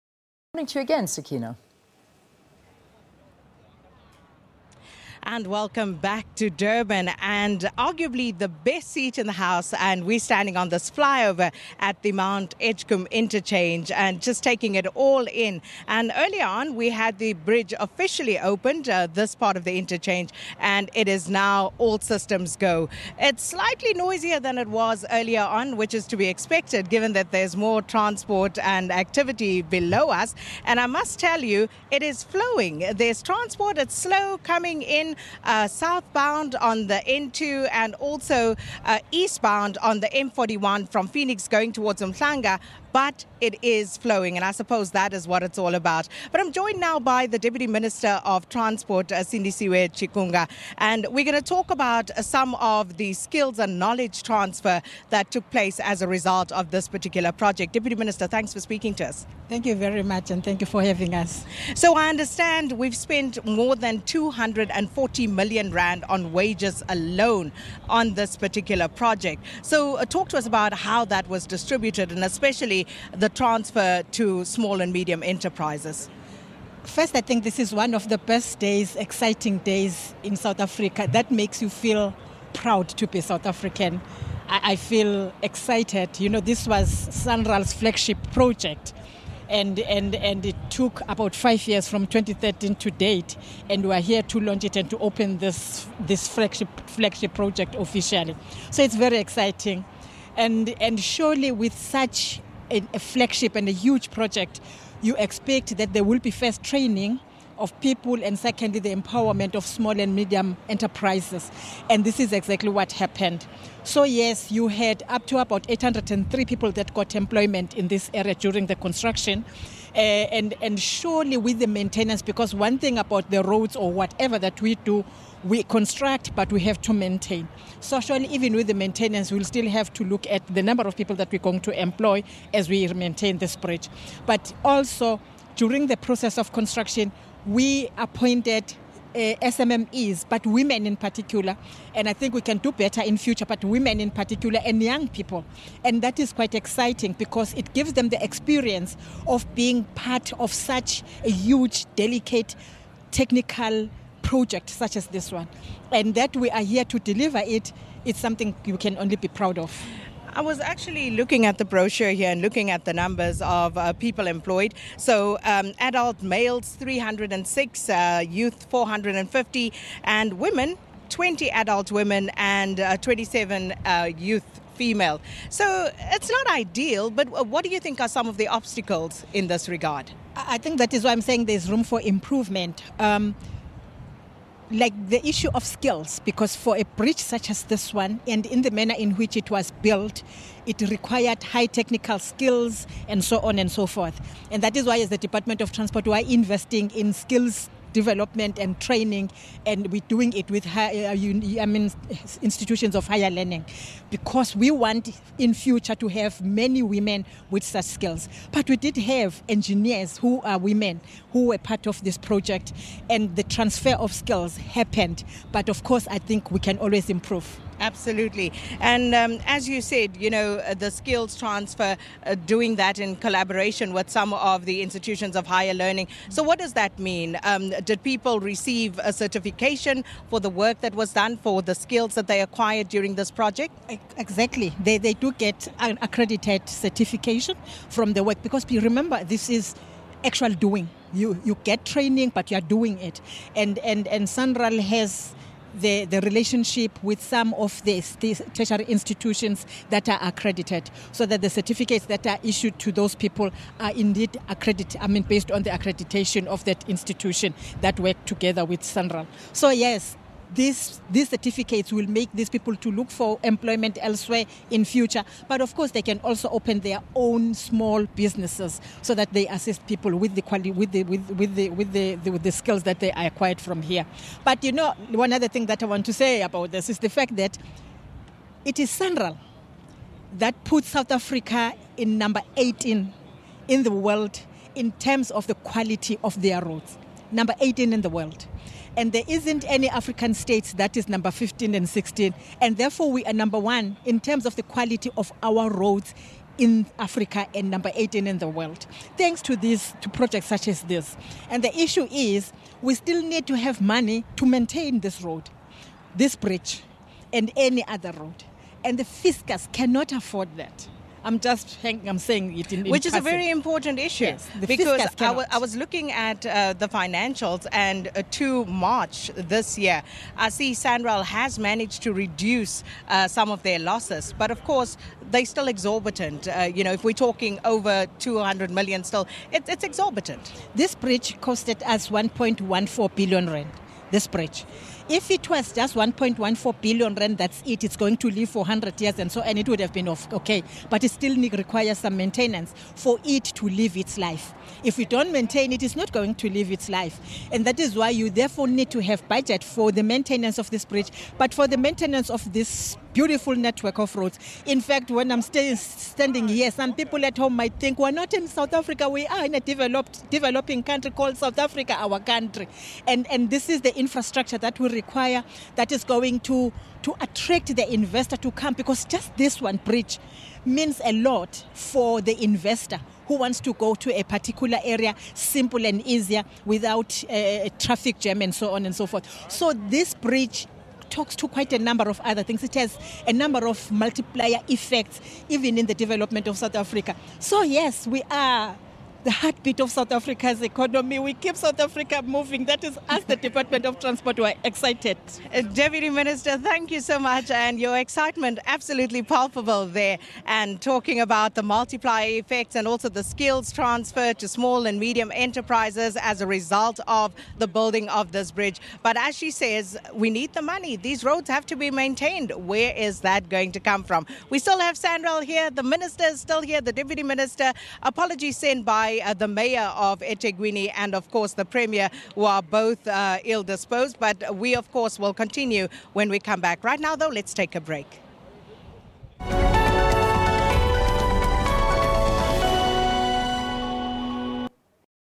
Sindisiwe Chikunga on World-Class Mt. Edgecombe Interchange
Sakina this morning is in Durban for the official launch of the Mount Edgecombe Interchange.